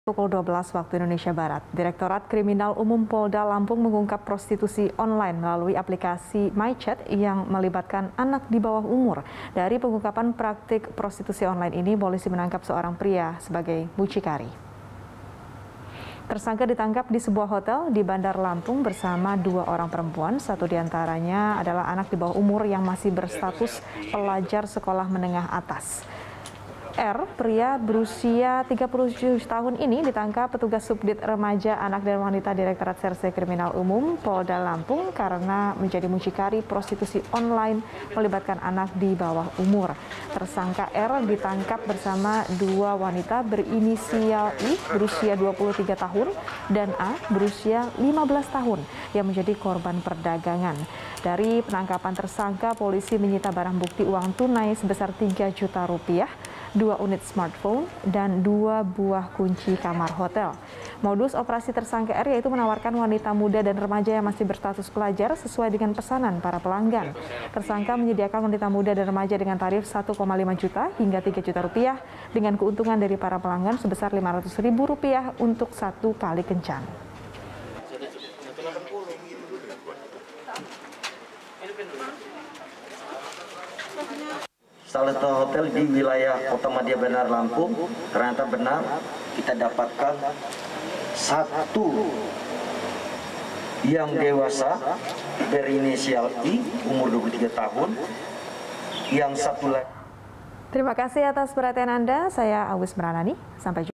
Polda Lampung Bongkar Prostitusi Online Anak di Bawah Umur
Direktorat Kriminal Umum Polda Lampung mengungkap prostitusi online melalui aplikasi MiChat yang melibatkan anak di bawah umur. Dari pengungkapan praktek prostitusi online ini, Polisi menangkap seorang pria sebagai muncikari.